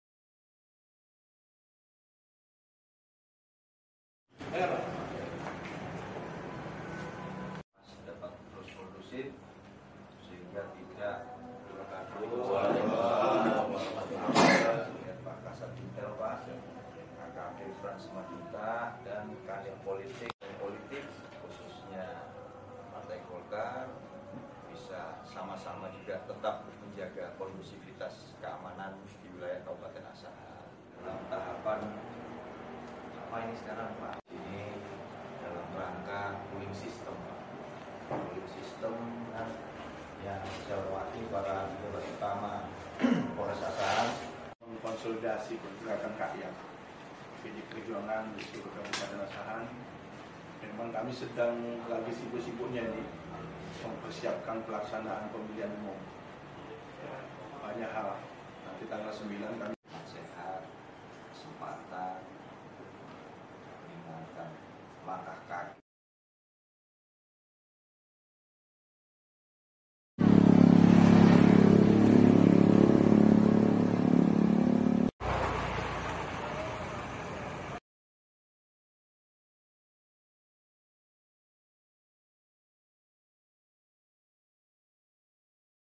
KAPOLRES ASAHAN SAFARI KAMTIBNAS KE 3 KANTOR PARTAI POLITIK
Kapolres Asahan AKBP Roman Smaradhana Elhaj gencar melakukan safari Keamanan dan Ketertiban Masyarakat (Kamtibmas) di Kabupaten Asahan.

Kali ini, Kapolres Asahan AKBP Roman Smaradhana Elhaj mendatangi 3 kantor partai politik yang berada di jalan Akasi, Kelurahan Mekar Baru, Kecamatan Kisaran Barat, Kabupaten Asahan, Selasa (7/2/2023).

"Ada tiga kantor partai yang saya kunjungi, yakni partai PDI Perjuangan, Partai PPP serta partai Golkar. Cooling System harus dilakukan agar suasana kondusif, aman, dan nyaman dapat terwujud di Kabupaten Asahan," terang AKBP Roman.

PANTAU TERUS BERITA TERUPDATE DARI KAMI

Instal Aplikasi Teras7.com di Google Play agar terus terhubung dengan kami